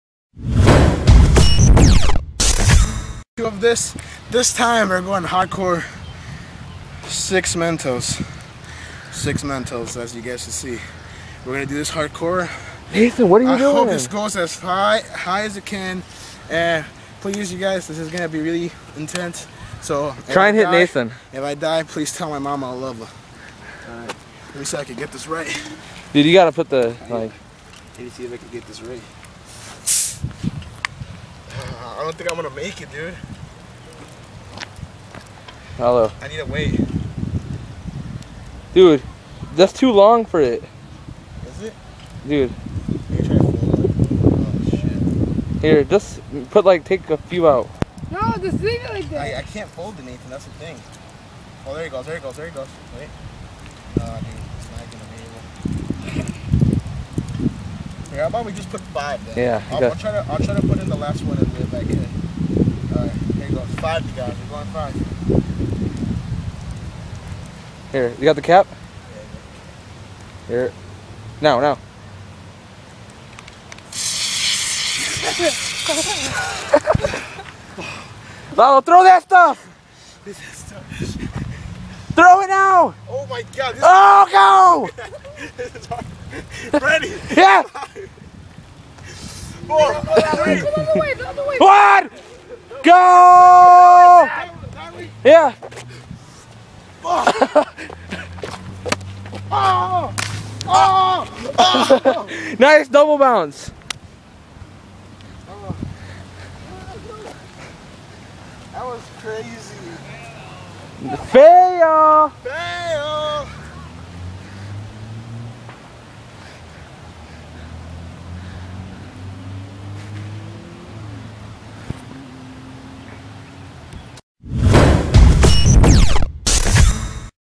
XxUNCATCHABL3xX Experements
coke+mentos=rocket
1.you need scotch tape
2.hard mentos not chewy
3.place 3 mentos on tape
4.put mentos in the diet coke and close it as soon as possible
5.shake diet coke
6.throw it as high as you can
7.watch it how high it goes